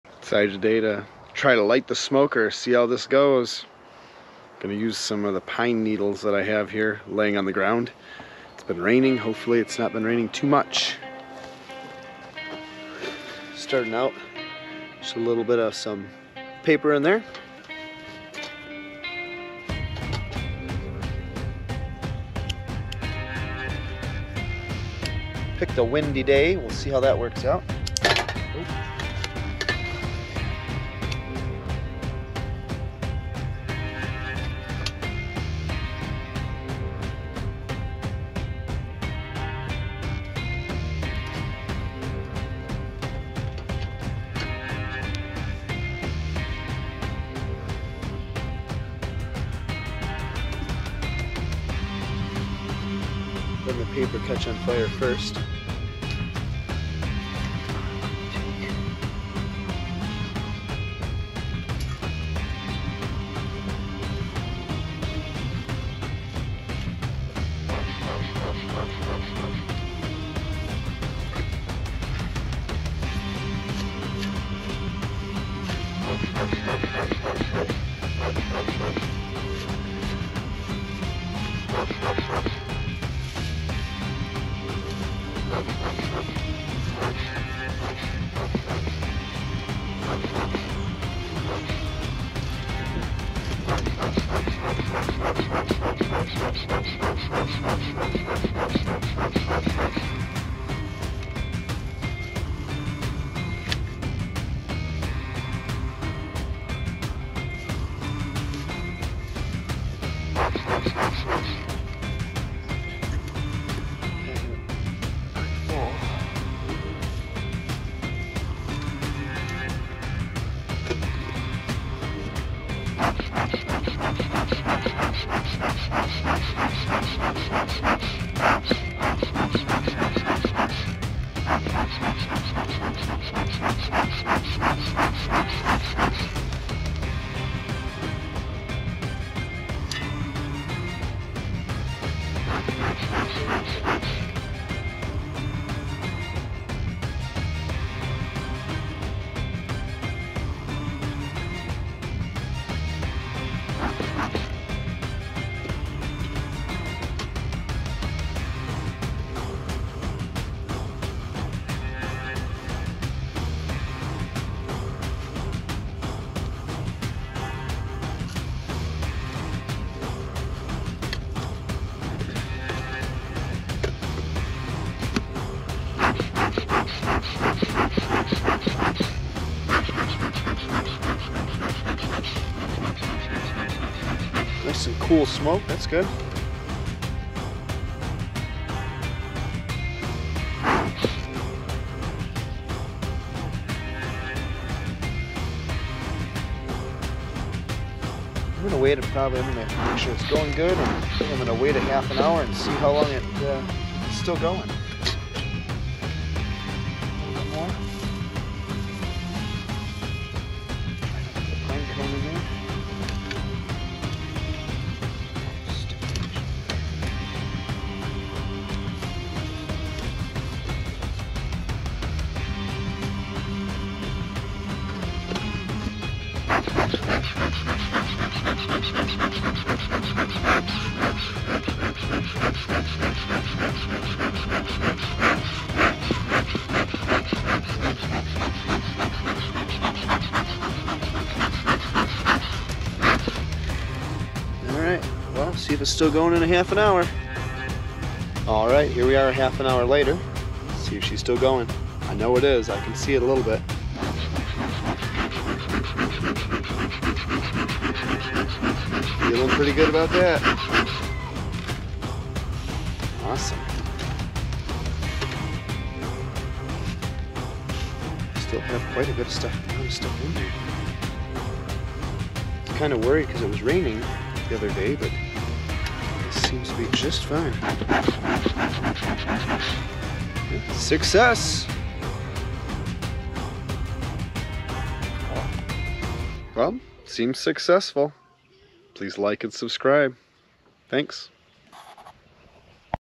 How to Light a Bee Smoker Using Pine Needles and Paper - Easy Beekeeping Tips
Learn how to light a bee smoker using pine needles and paper! 🐝🔥 In this video, I'll show you a simple and effective method to get your bee smoker going, ensuring a smooth beekeeping experience. With recent rain, I wasn't sure if this would work, so let's find out together. Lighting the smoker is an essential skill for beekeepers, helping to calm the bees by masking the scents in the hive.

What You'll See:
Bee Smoker Setup: Step-by-step guide to lighting a bee smoker using pine needles and paper.
Free Smoker Fuel: Discover the best natural materials for bee smoker fuel, including hay, grass clippings, and pine needles.
Practical Tips: Learn about other potential fuel sources like old cotton t-shirts, jeans, burlap bags, and sisal baling twine.
Why You'll Love This Video:
Educational Content: Gain valuable knowledge on a key beekeeping skill.
Easy Method: Follow along with a simple and effective method to light your bee smoker.
Beekeeping Insights: Learn practical tips to enhance your beekeeping experience without spending money on commercial smoker fuels.

Timestamps:
0:00 - Introduction
0:12 - Lighting the Smoker
4:08 - Half Hour After Lighting (No Pumping)
4:45 - Conclusion and Subscribe